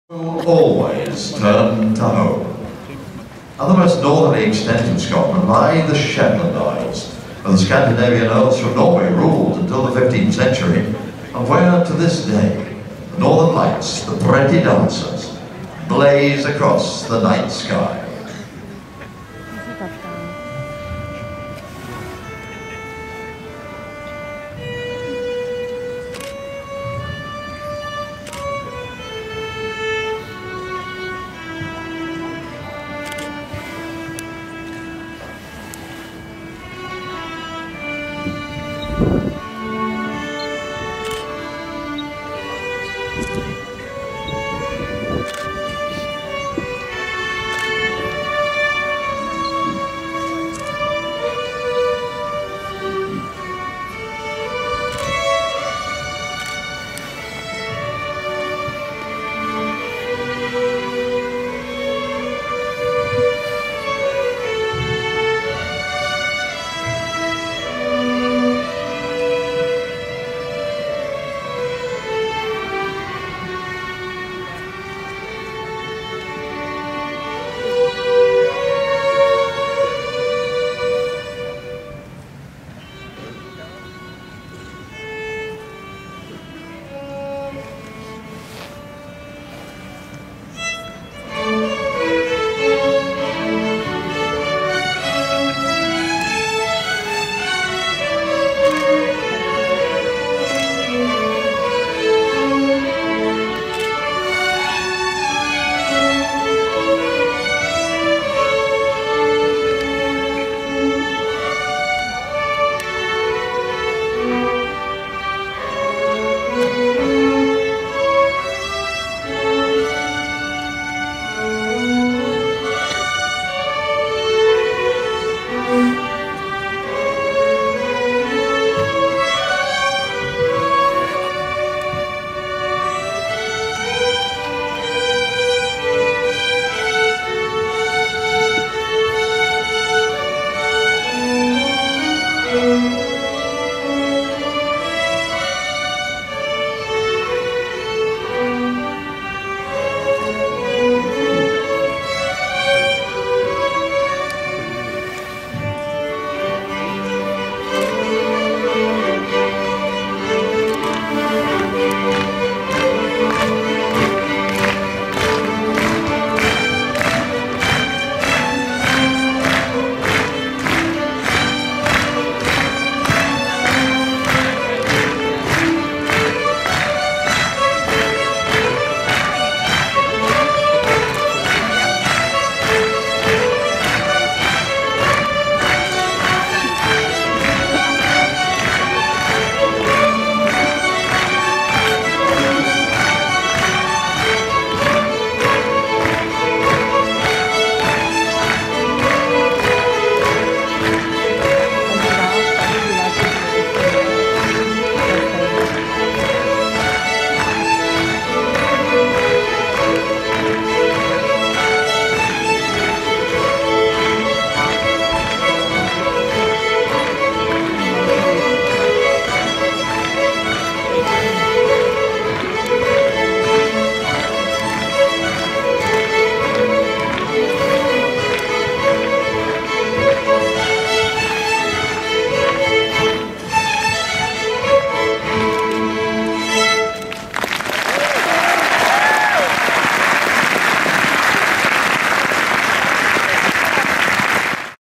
Royal Edinburgh Military Tattoo 2014 Shetland Fiddlers
If you are in Edinburgh, Scotland in August, please try to see this performance!  Should not be missed!